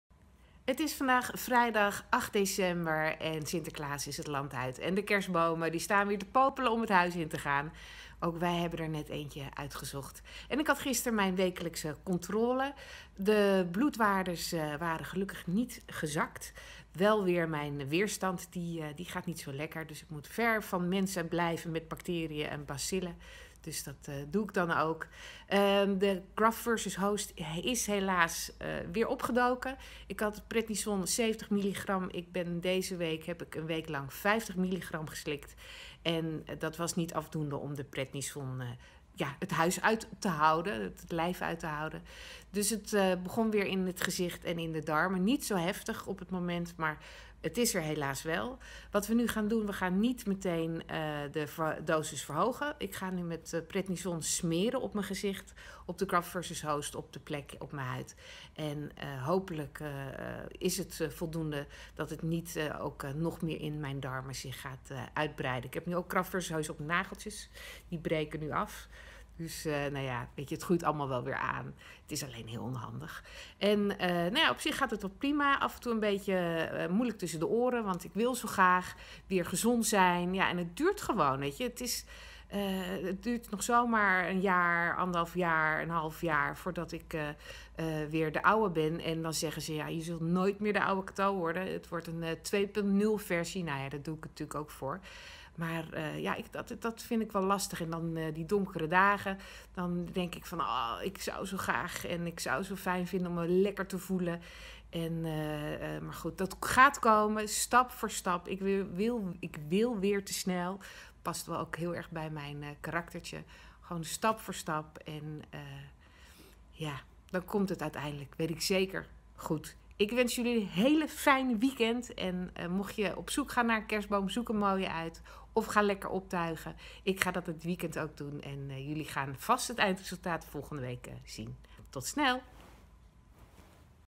VLOG 42 Graft vs Host pop weer op. Leukemie Stamceltransplantatie
Vlog 42 Cato-Margo van Rheenen Peekel in de achtbaan die Leukemie heet. Nu van 70 mg prednison naar 50 MG gegaan. Helaas popt de Graft vs Host (de donor stamcellen vallen mijn lijf aan) toch weer op. Nu extra prednison smeren op de huid en open dat het in de darmen stabiliseert. Nu ook gaft vs host in mijn nagels. Ze brokkelen af tot aan het vel. Beetje pijnlijk maar zal tzt wel weer mooi aan groeien. Ik wil sneller beter worden dan mijn lijf aan kan. Dat frustreert maar hoort ook bij het proces van stamceltransplantatie. Zou zo snel mogelijk weer de oude Cato-Margo willen zijn. Artsen zeggen dat je nooit meer de oude Cato-Margo wordt maar een 2.0 versie er van. Stap voor stap dat is de enige manier maar ik zou zo graag wat stapjes willen overslaan. Normaal is mijn suiker 5 en nu plots 10. Dit kan een bijwerking zijn van de prednison. Deze week af en toe even prikken om te kijken of het zakt of juist stijgt.